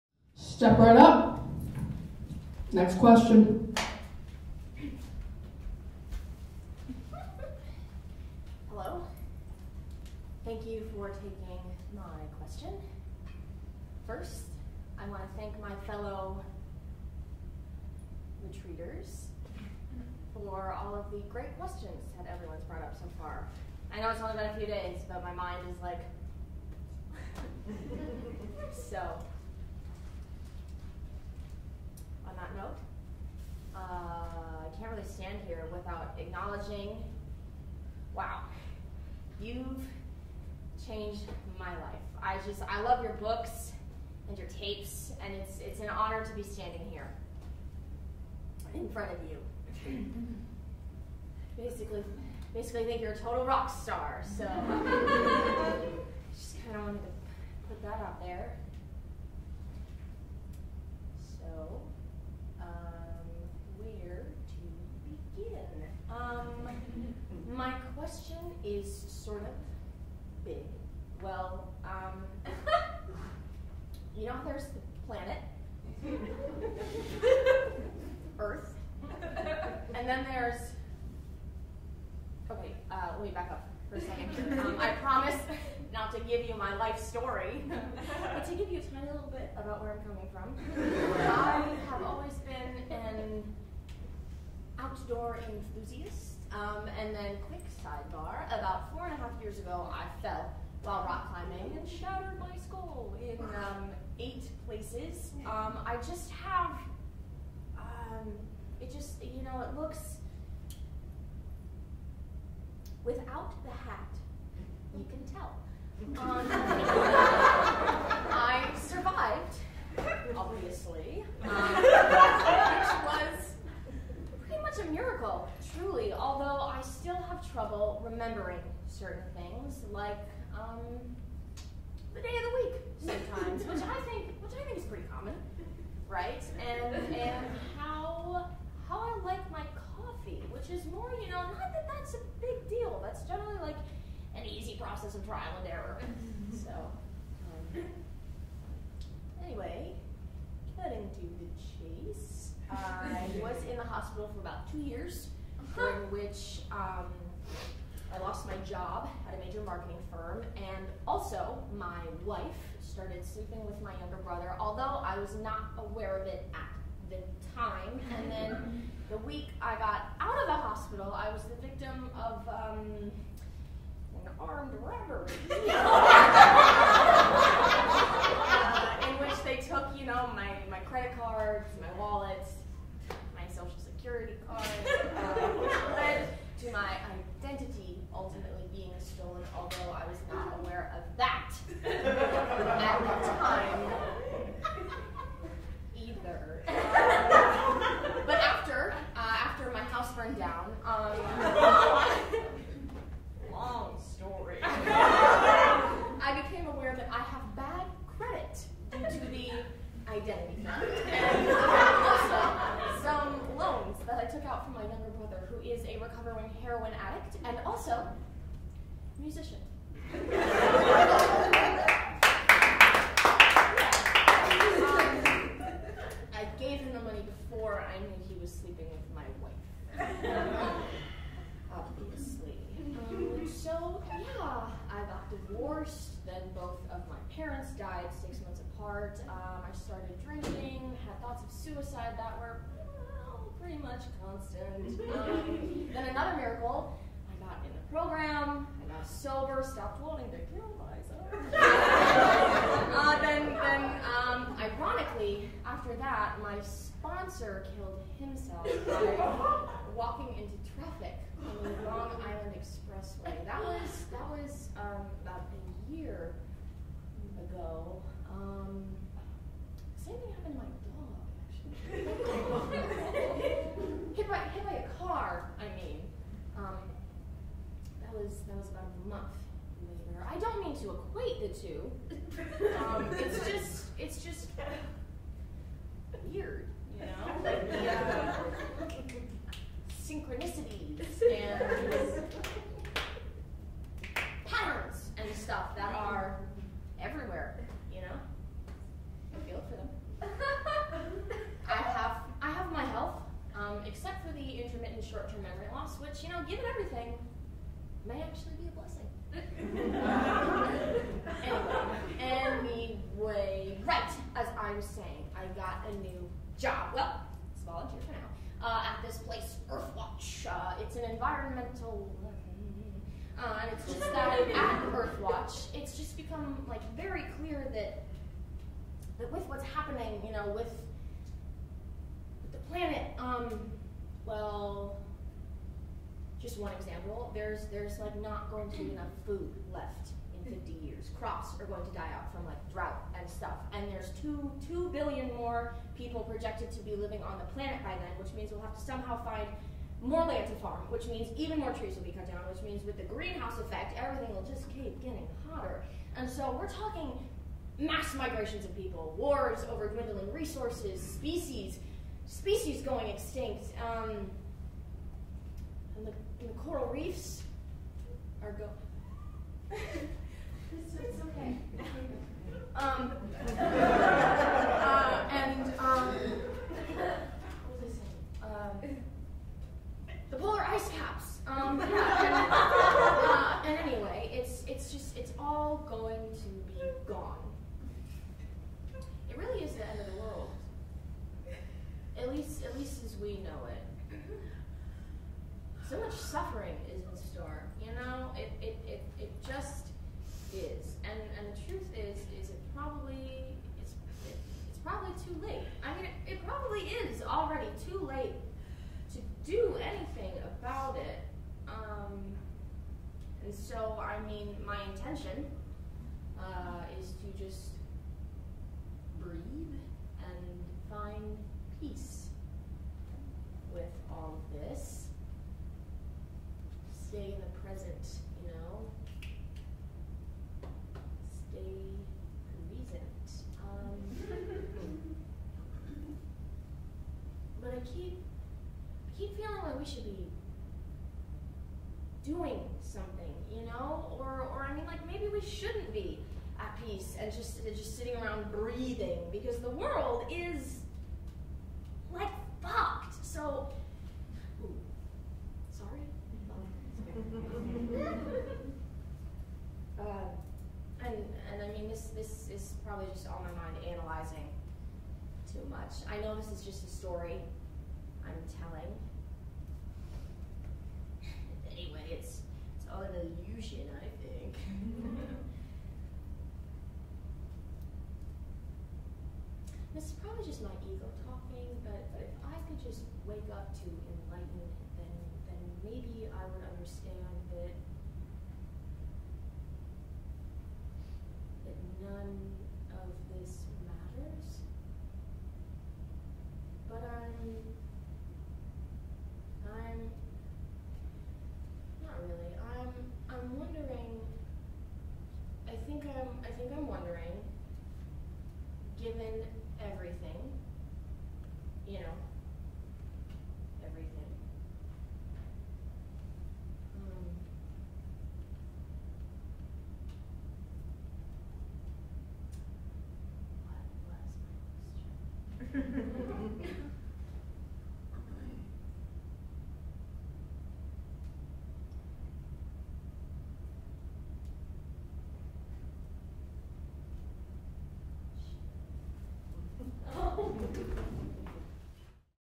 Small Mouth Sounds | Ned's Monologue
Bella West as Ned in Boston Conservatory's Production of Small Mouth Sounds Directed by Peter Toto